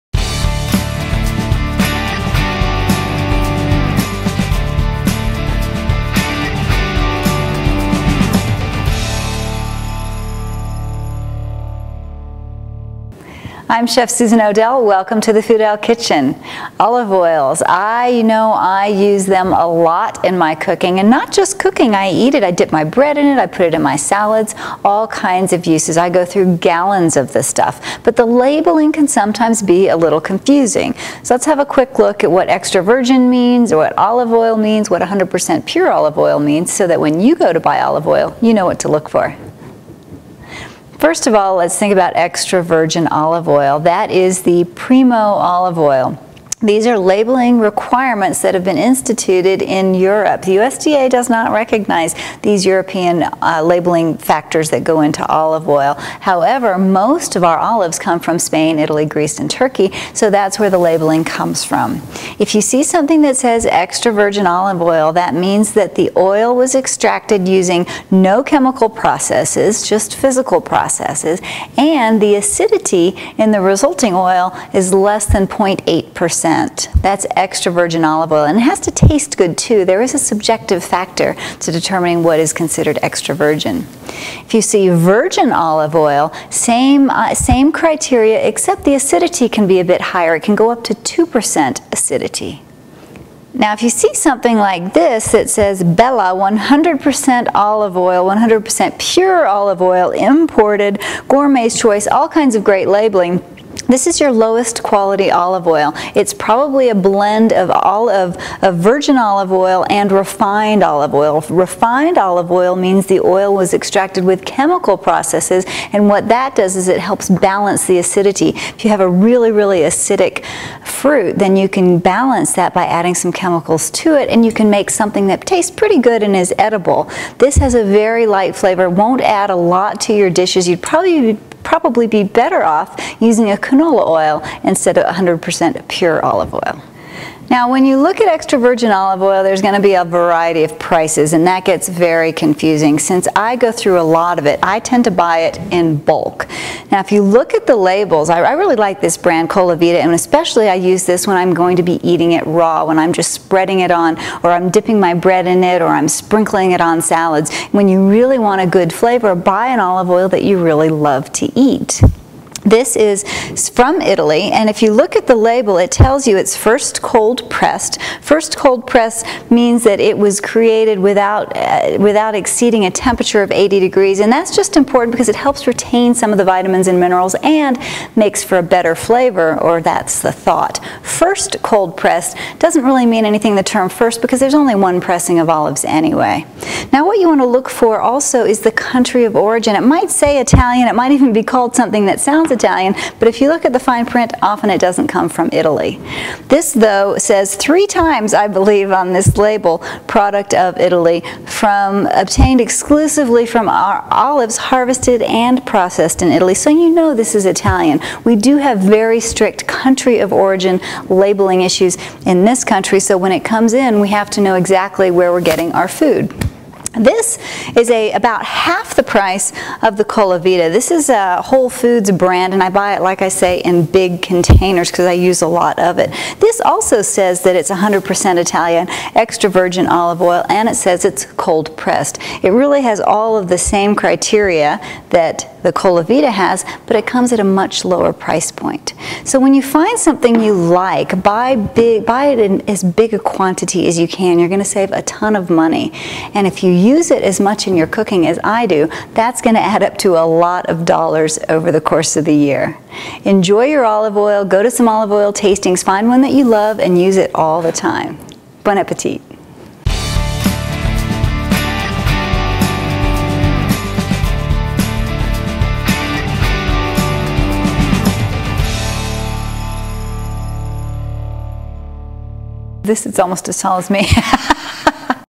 Olive Oil
Olive oil has many great uses - cooking, salad dressings and dipping bread are just a few.  Learn what the different types of olive oils are so you make good decisions when buying and using olive oil.  Chef Susan Odell of foodell.com explains it all in this video.  foodell.com is your online cooking school.